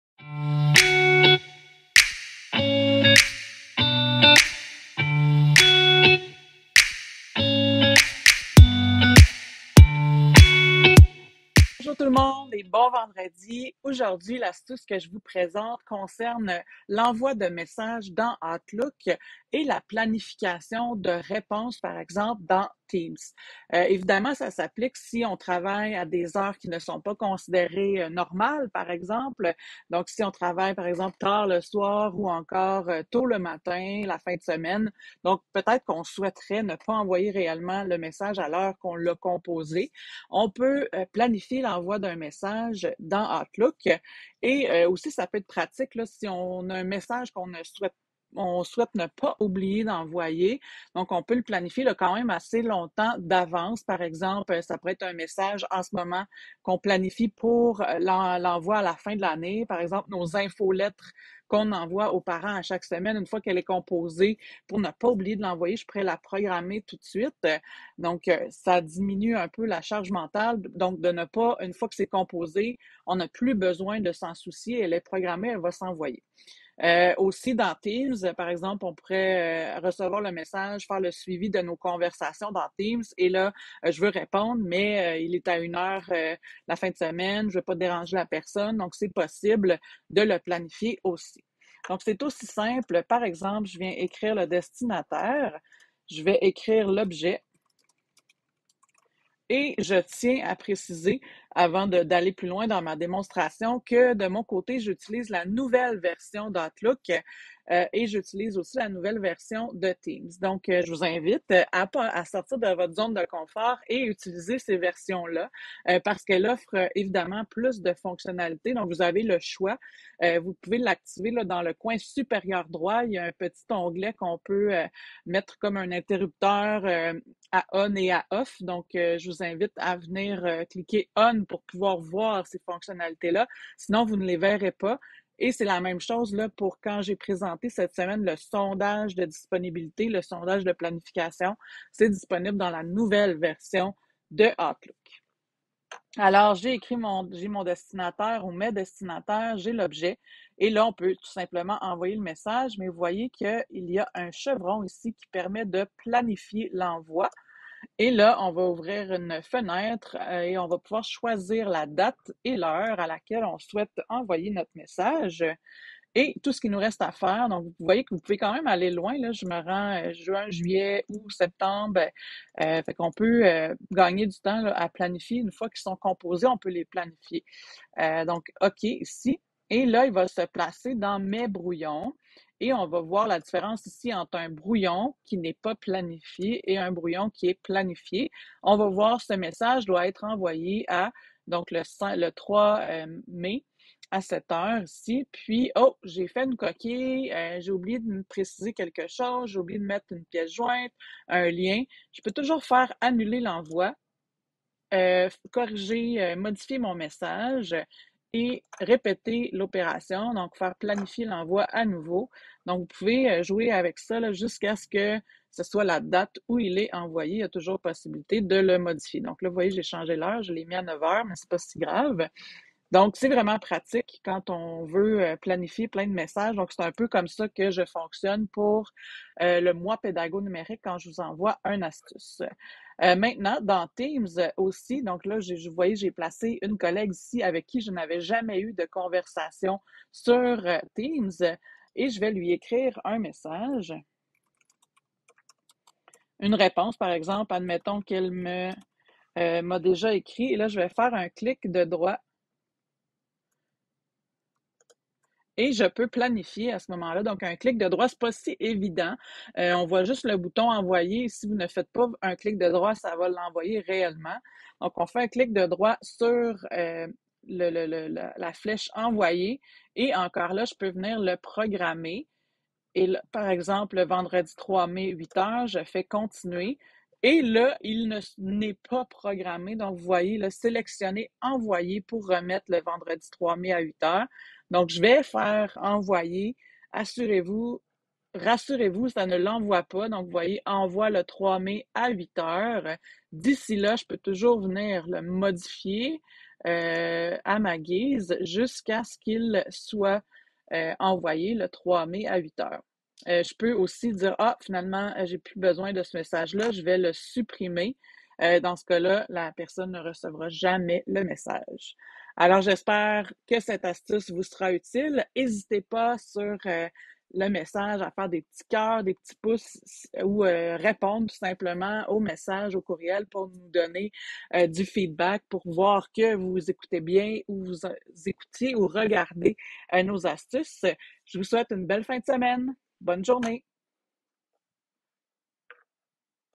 Outlook et Teams Planifier l'envoi d'un message
La planification de messages dans Outlook et Teams. Gagnez en efficacité en planifiant vos communications à l'avance, en évitant les oublis et en optimisant votre temps.